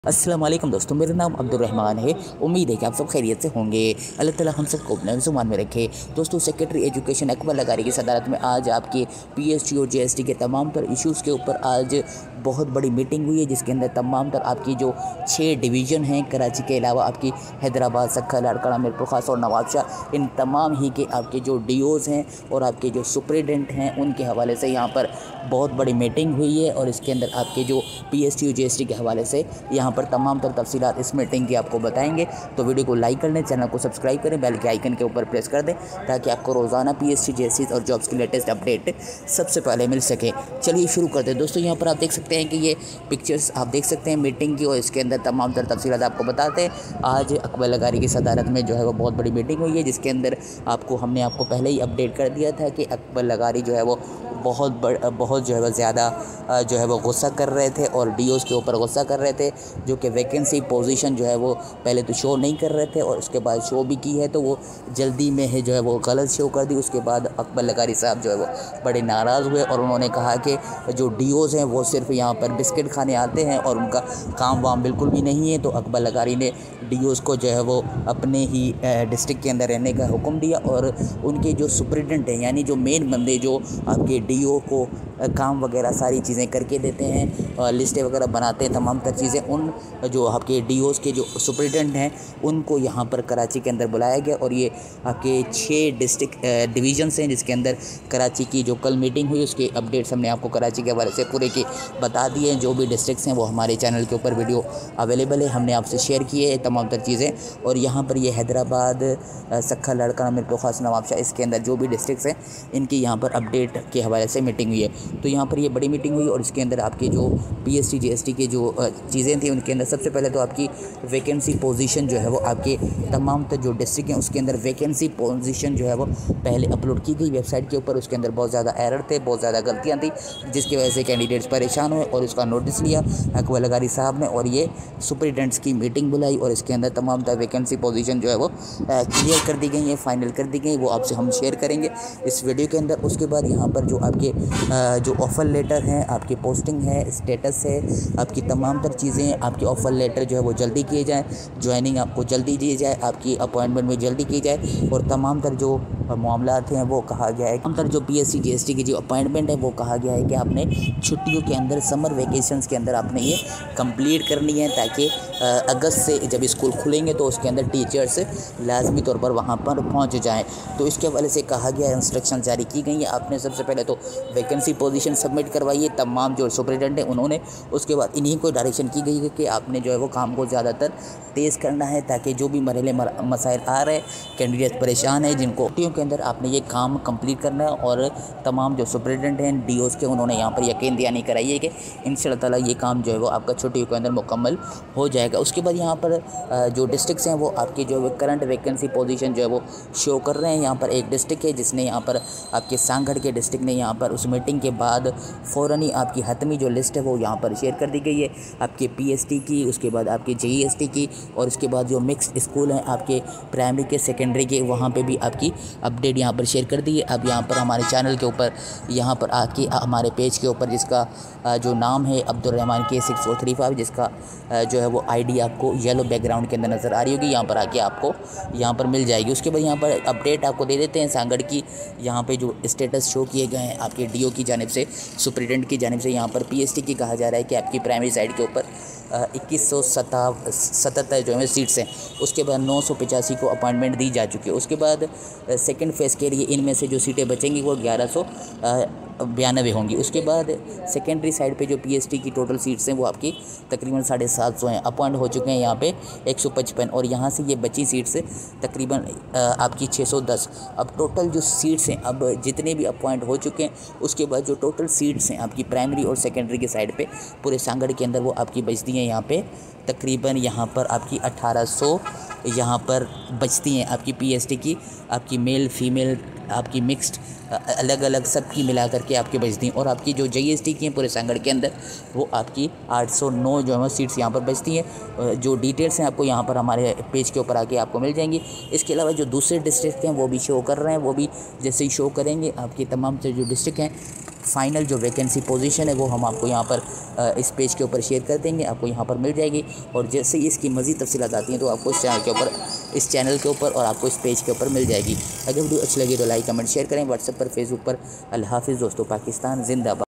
AKBAR LAGHARI today PST JEST meeting update - PST JEST vacancy position - pst jest offer letter news
Education minister Saeed Ghani Announcement for PST & JEST Test Date| IBA PST TEST | JEST IBA TEST